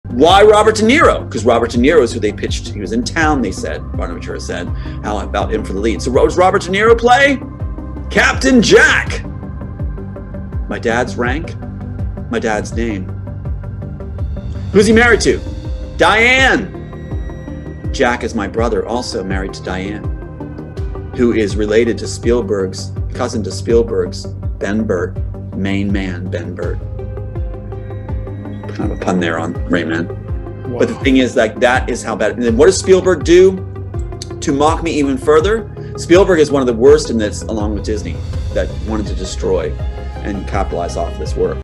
"Robert DeNiro as Neo, Dustin Hoffman as Architect" Tom Althouse Clip 049
@yissilmissil Interview w/ Tom Althouse - Creator of "The Immortals" (aka "The Matrix")